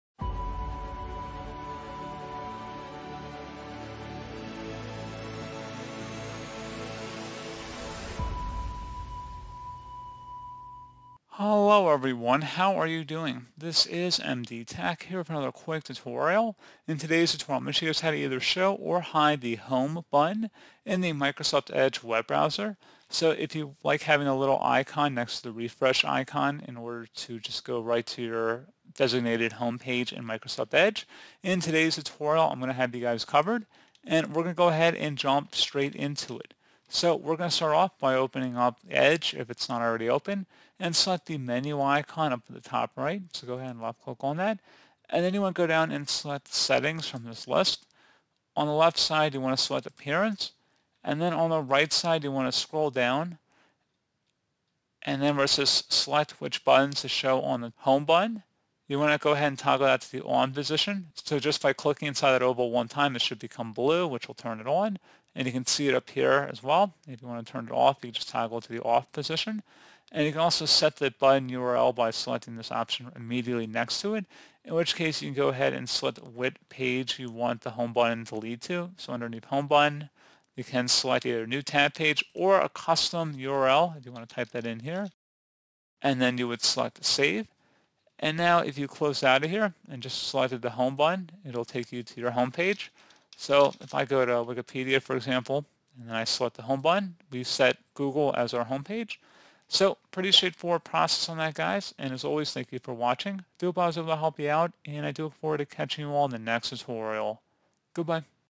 How to Show ‘Home Button’ in Microsoft Edge [Guide]
Microsoft Edge web browser in Windows 11/10 now lets you add a Home button. While some of you may not find any use for the Home button, others like to have a Home button visible on the browser interface. When you click on this Home button, it will open the page you set it to open – maybe even your favorite website.

The Home button in a browser is an icon found on the toolbar of the Microsoft Edge. This button will redirect the user back to a predetermined homepage. Depending on the user’s system usage, they may want to add or remove this button. By default, the Home button will be disabled. Users can add the Home button from one of the methods in this tutorial.

In the method outlined above, we have also included steps through which users can remove the Home button. It also included steps that will disable the Home button settings from standard users.